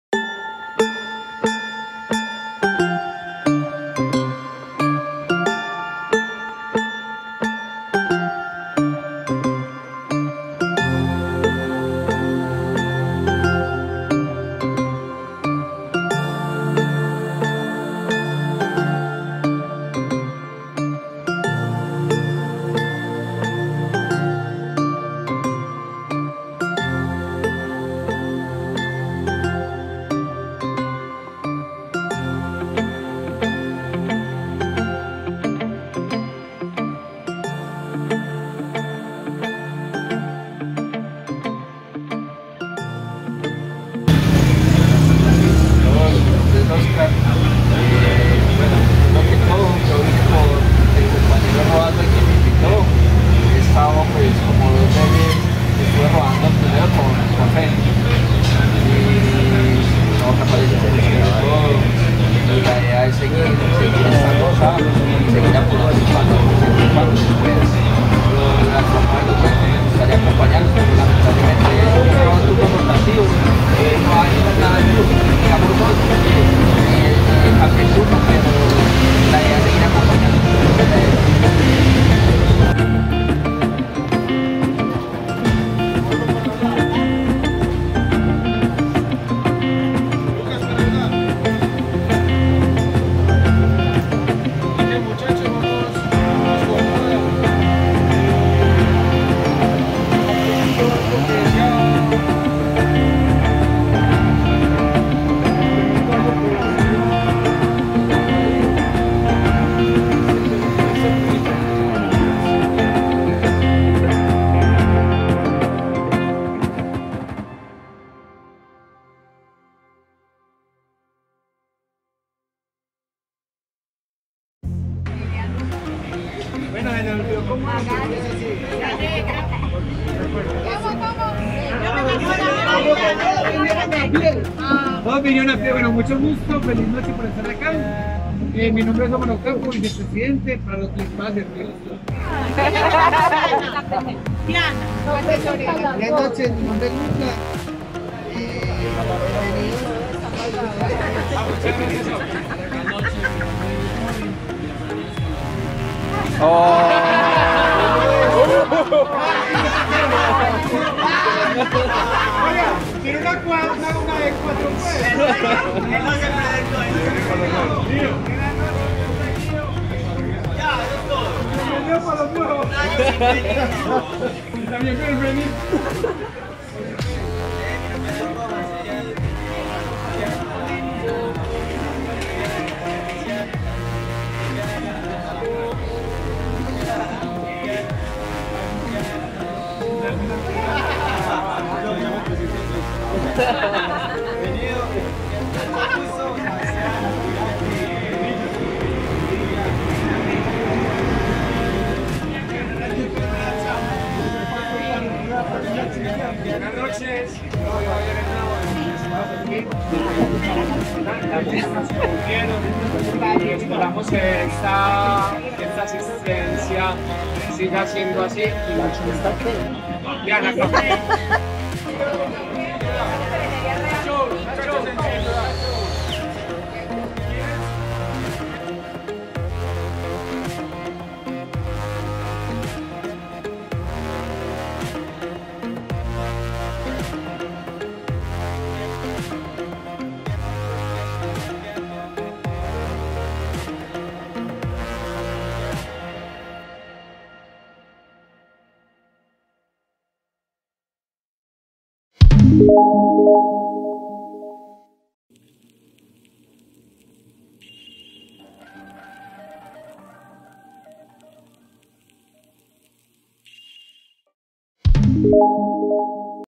REUNION DE MOTEROS
Un saludo a todos (A) los invito a ver el siguiente video, presentacion de moteros,planificacion de rodada, metas y objetivos.
"Recuerda apoyarme " dando like, gracias totales.